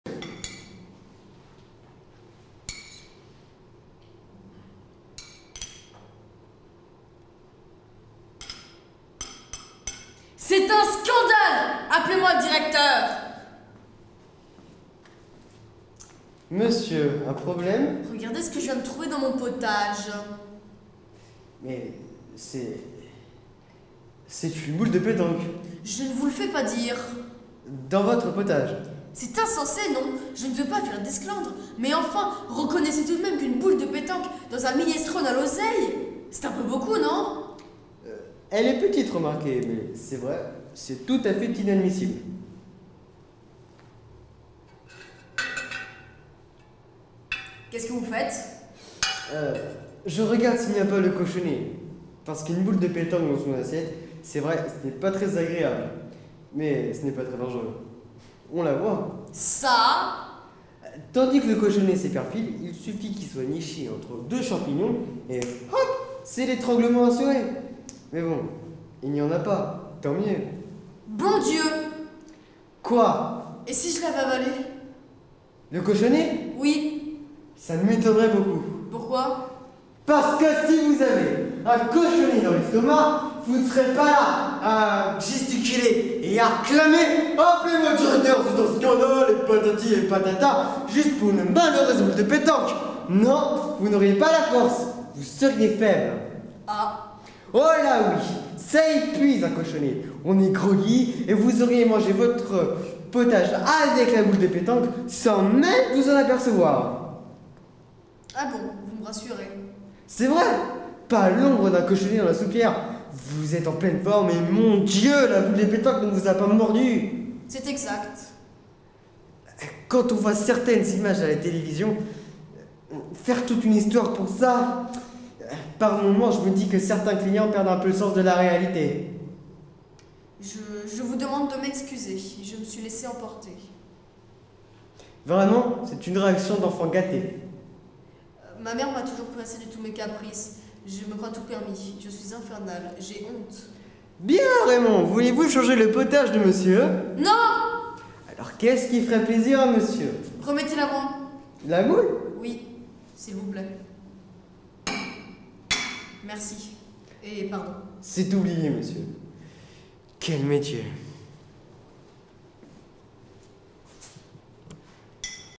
La boule de pétanque
atelier Acteur Caméra.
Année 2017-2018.
Cours de Dominique Le Guennec qui se déroulent toute l'année au 108 rue de Bourgogne à Orléans.